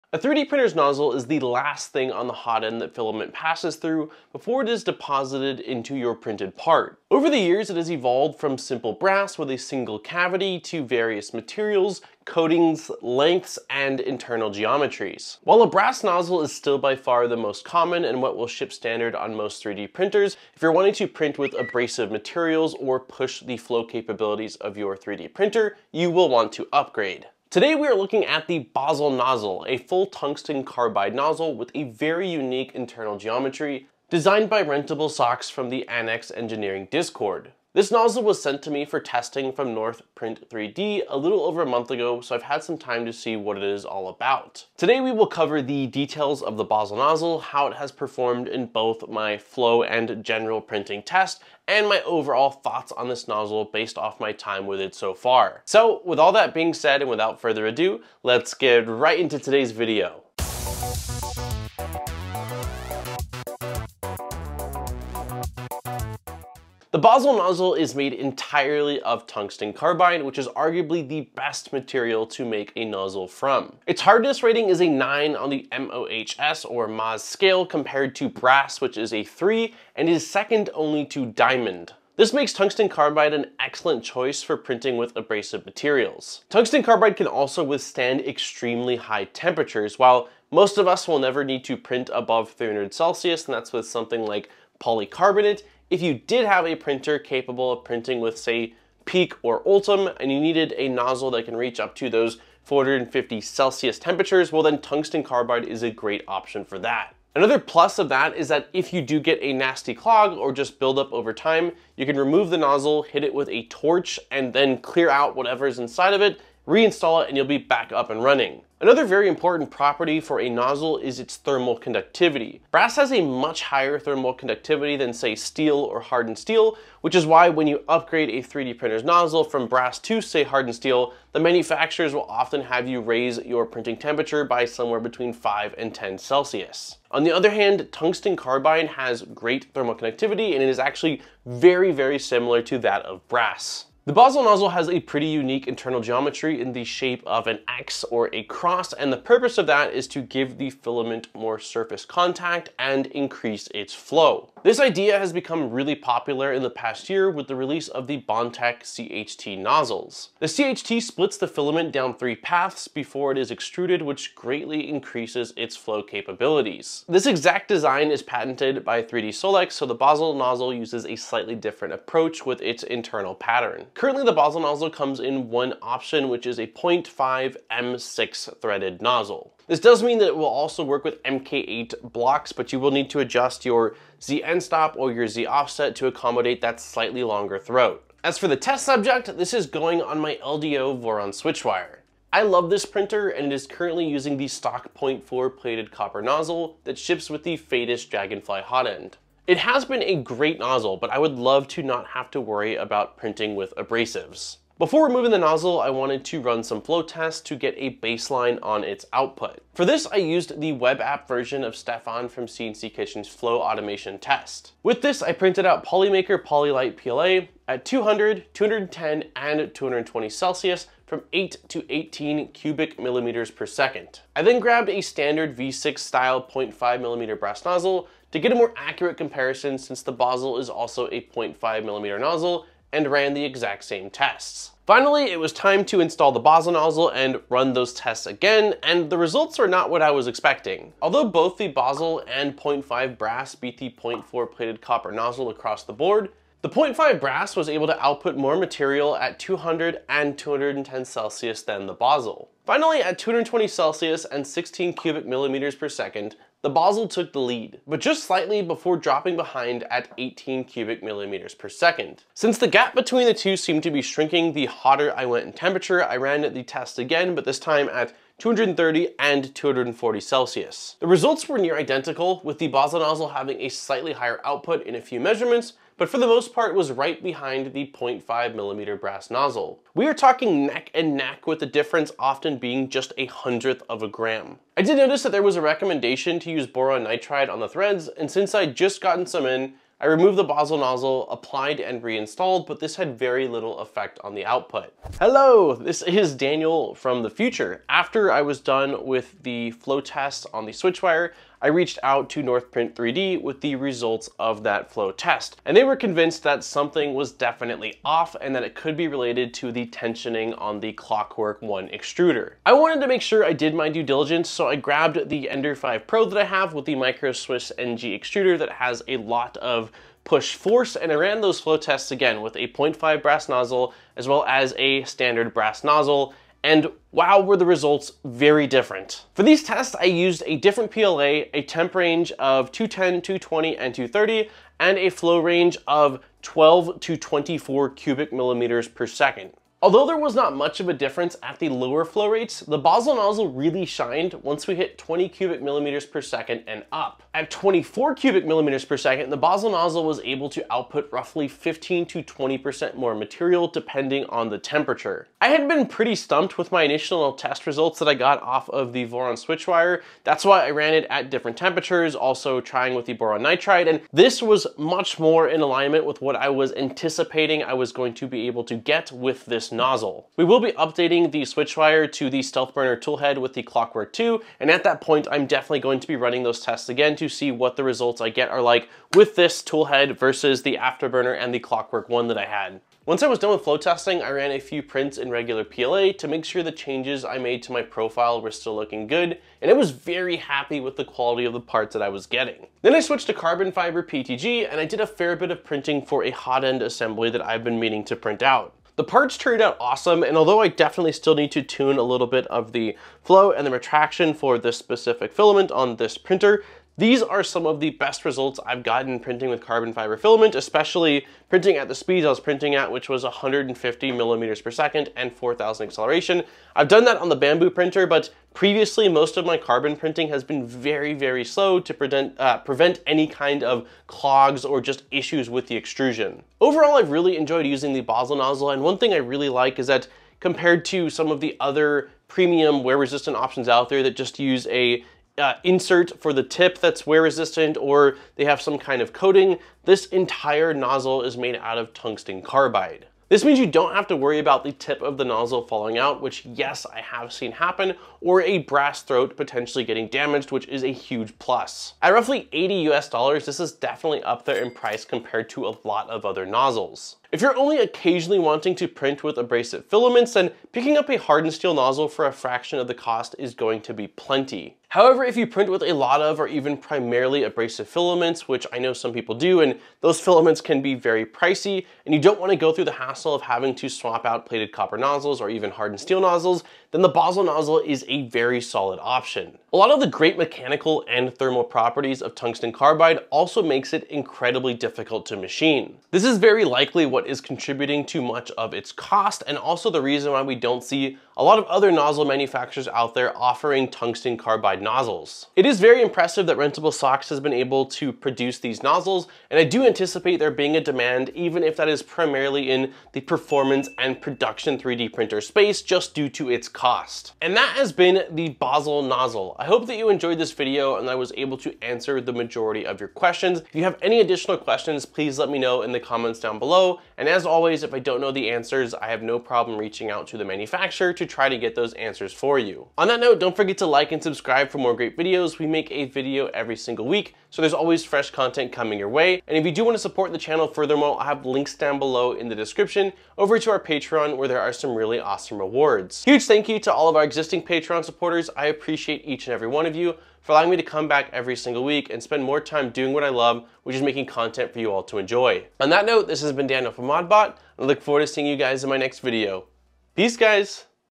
What Is A Bozzle Nozzle?
Find out more or purchase the Bozzle Nozzle here: (Price is in CAD)

In today's video we dive into the Bozzle Nozzle. A full tungsten carbide nozzle with a very unique internal geometry to provide higher flow. We will cover how it prints, what my flow rate results where like and my overall thoughts on this nozzle.